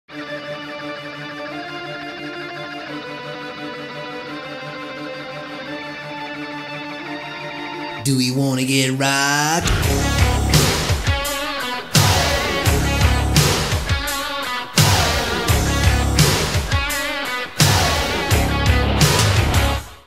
the Australian Def Leppard Show Coming Soon
Some of the best musicians in Sydney Australia have come together to this tribute to the songs and sounds of Def Leppard.